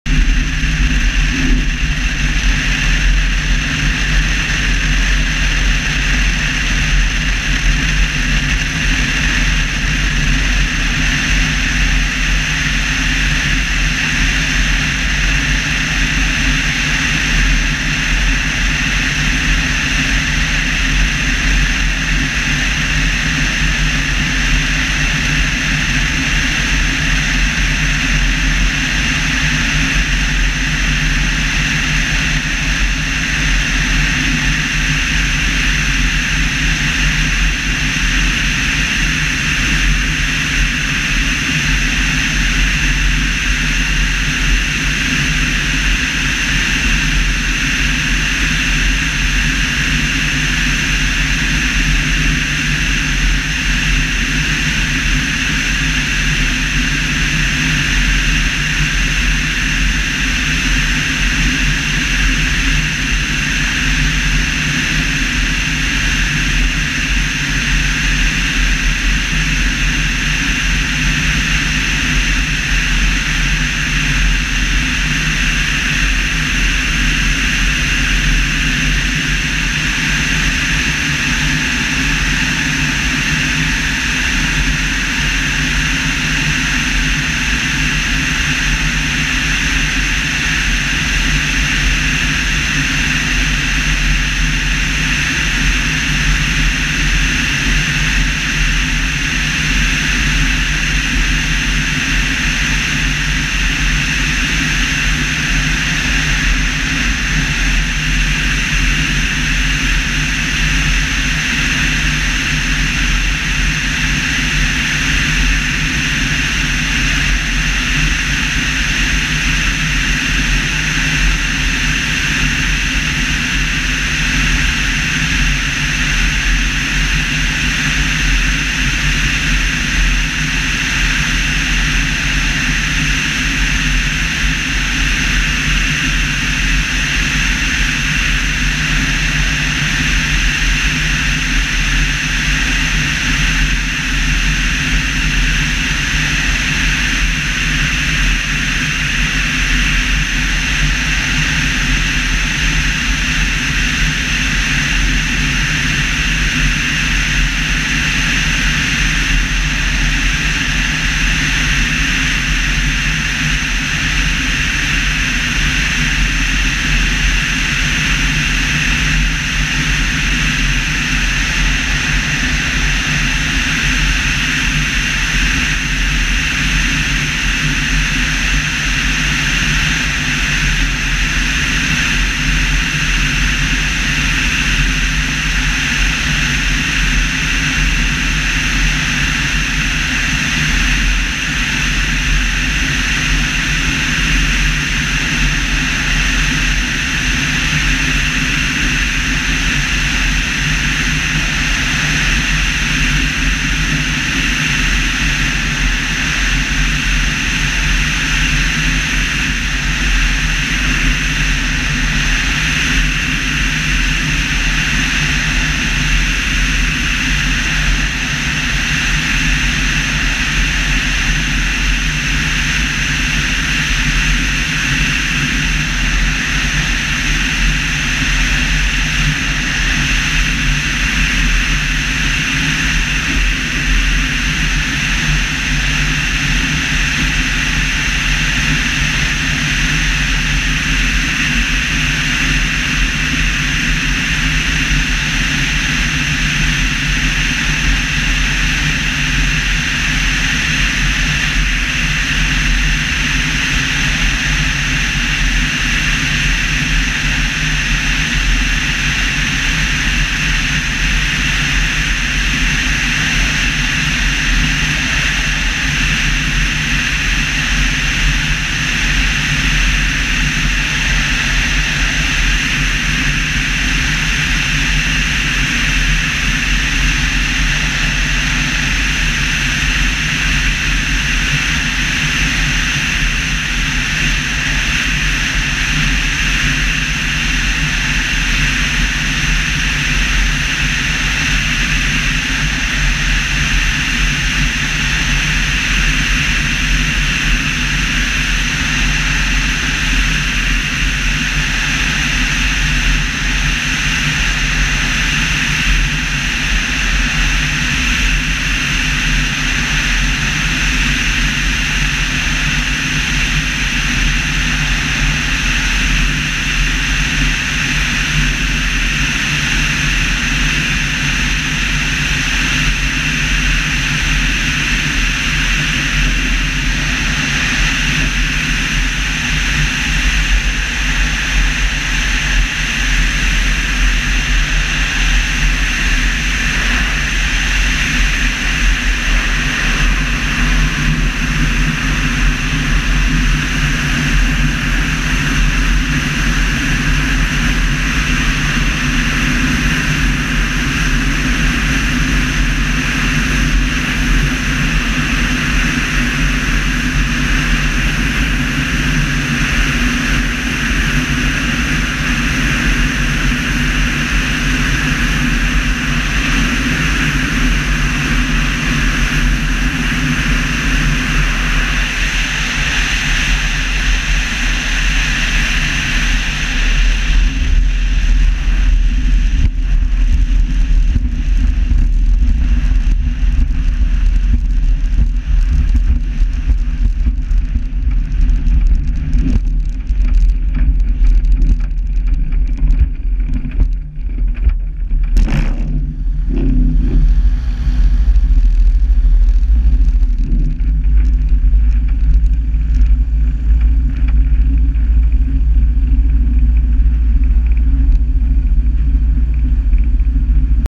Boston Deadstick
Evektor EV-97 Eurostar. Dead stick evening landing at Boston airfield in Lincolnshire UK. Filmed with my GoPro 3+ Black in 1080 48 frames.